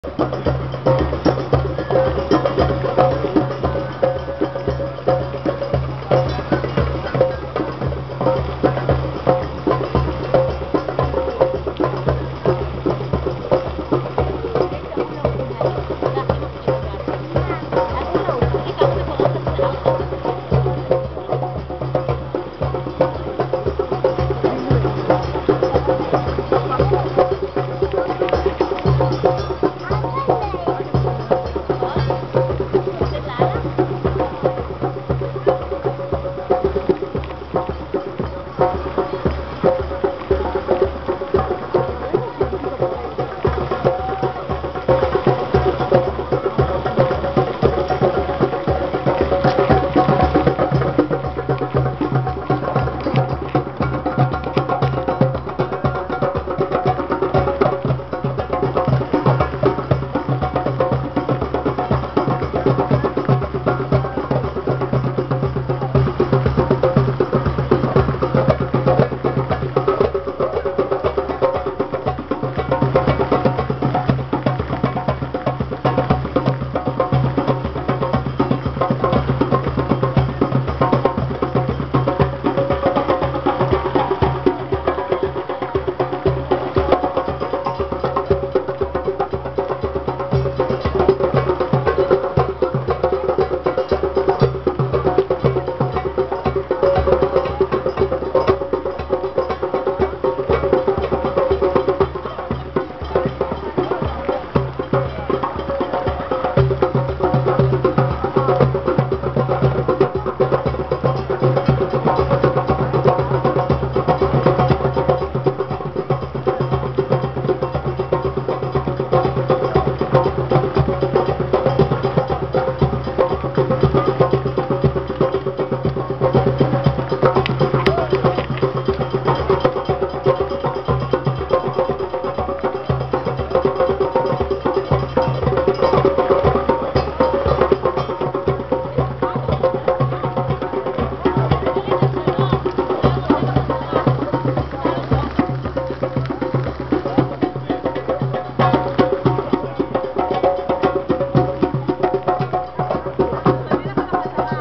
MOV05586.AVIHolahoop fire dance by israelies group.
I shoot this video in Telaviv Israel.in august 2011.the location was a circle fountain near Tandoori Restaurant(indian restaurant) TELAVIV.